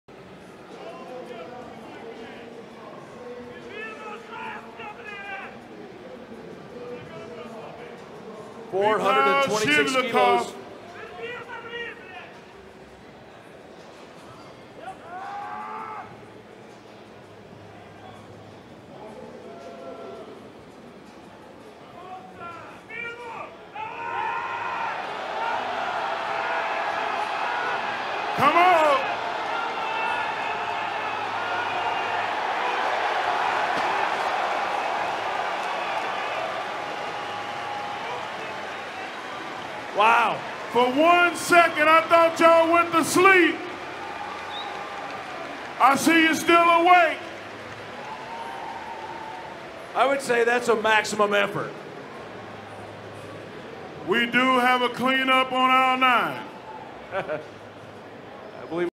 Mikhail Shivlyakov - 426kg 939lbs deadlift at 2018 Arnold Strongman Classic
he even starts to nosebleed